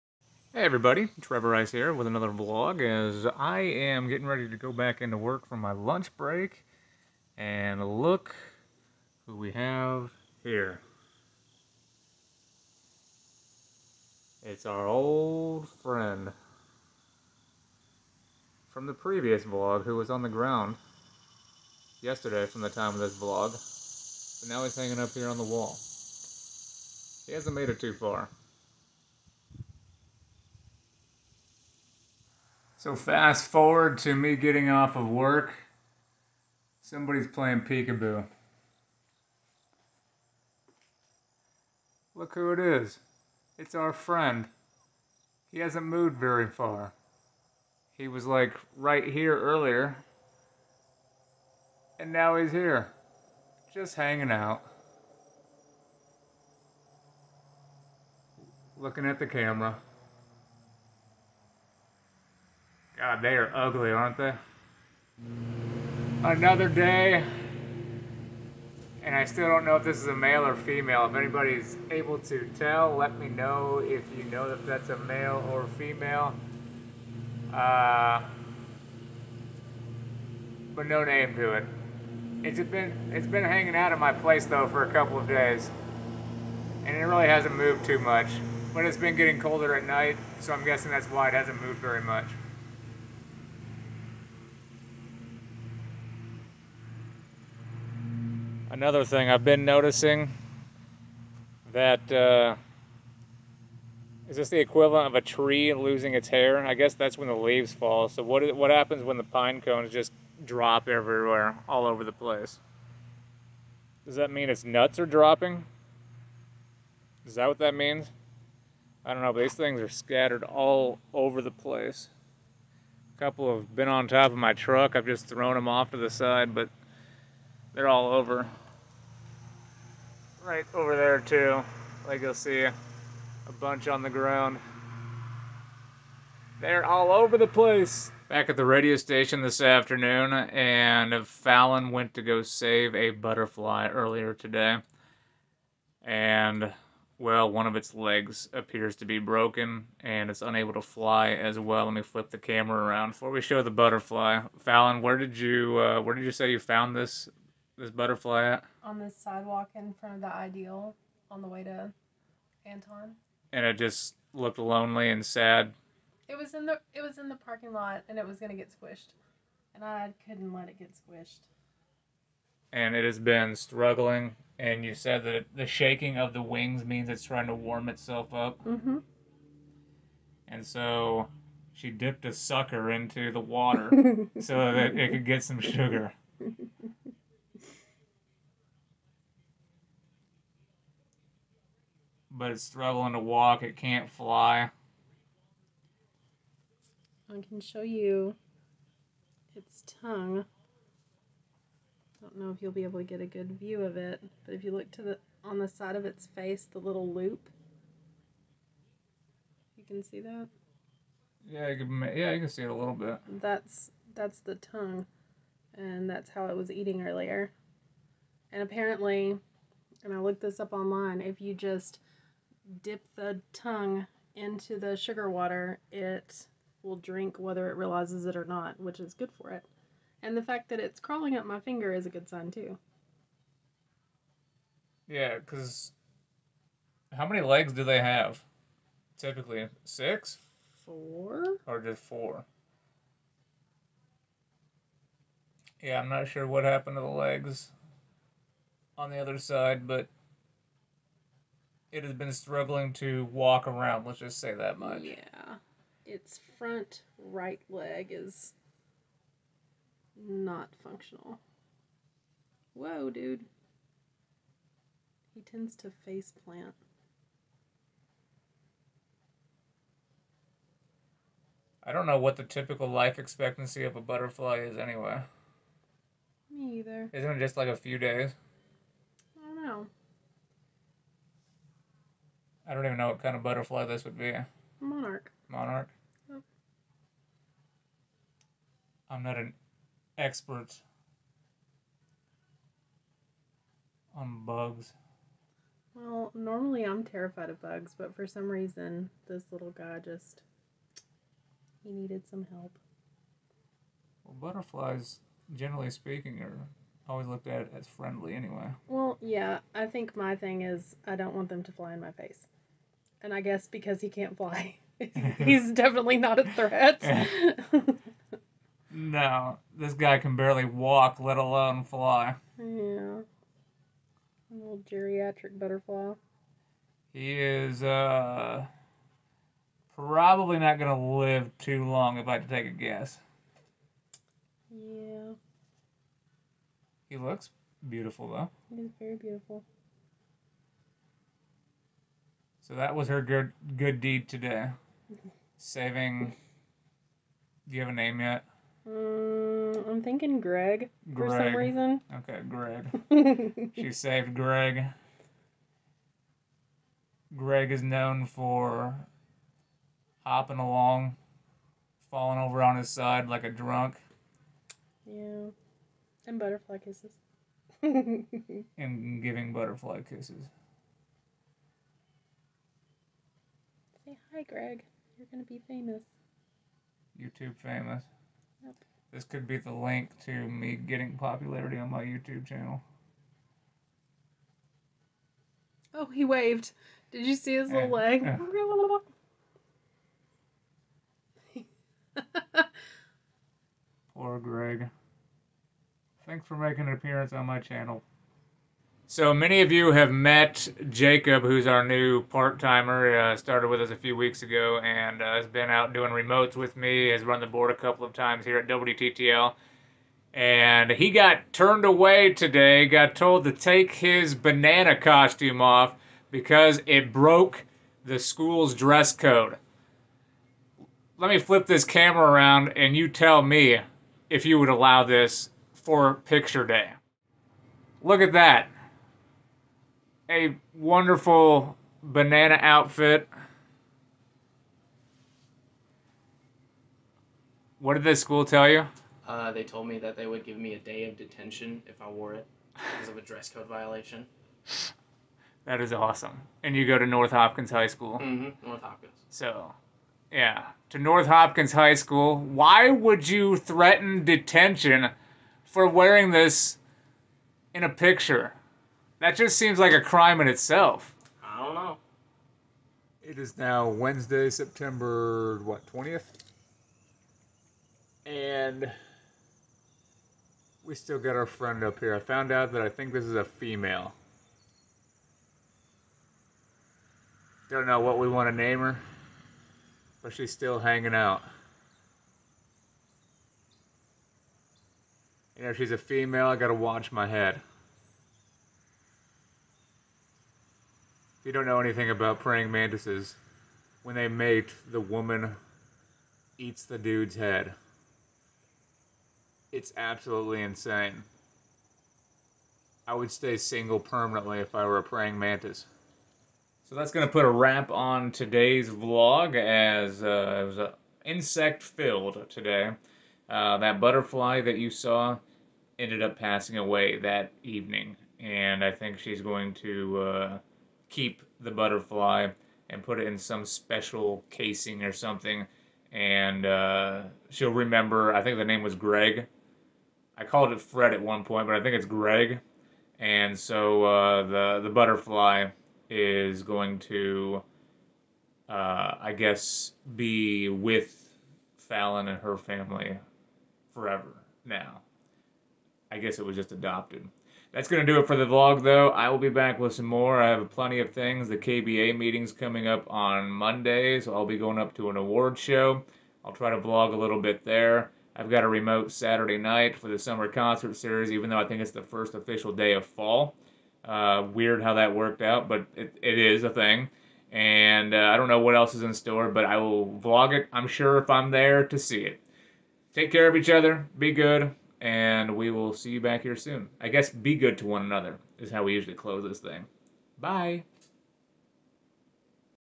A praying mantis wants my head.
There's a praying mantis hanging out at my place. Greg, the butterfly in the video unfortunately passed away the evening of the clip I took in this vlog video.  Also, Jacob was forced to take his banana outfit off, because it broke the dress code at the school.  I may have laughed.

@radiotrevorrice on Instagram and Twitter